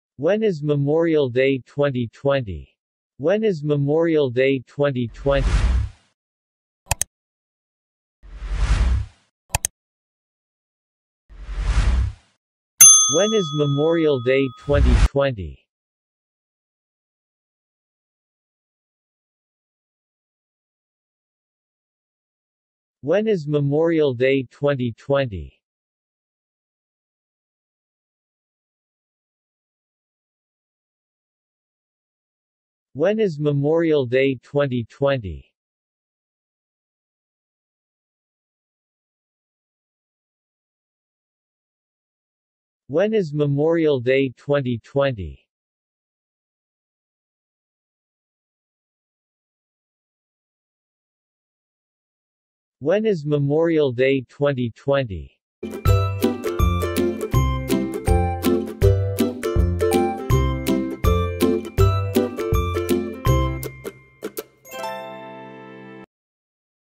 Learn How To Pronounce When is Memorial Day 2020
Memorial Day is a federal holiday in the United States for honoring and mourning the military personnel who had died while serving in the United States Armed Forces. Memorial Day is on Monday, May 25, 2020.

 When is Memorial Day 2020n